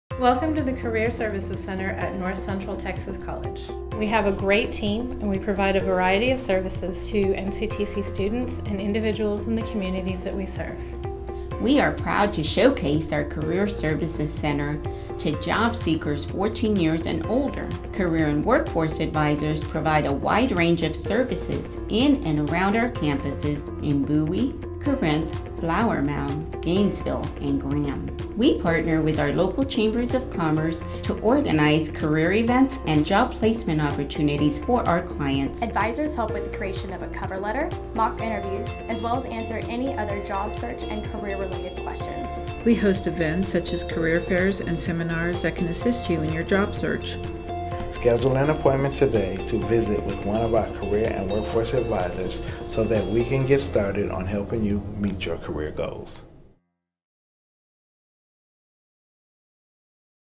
North Central Texas College - Career Services Center
A promotional video for the North Central Texas College Career Services Center.  Produced by MMC.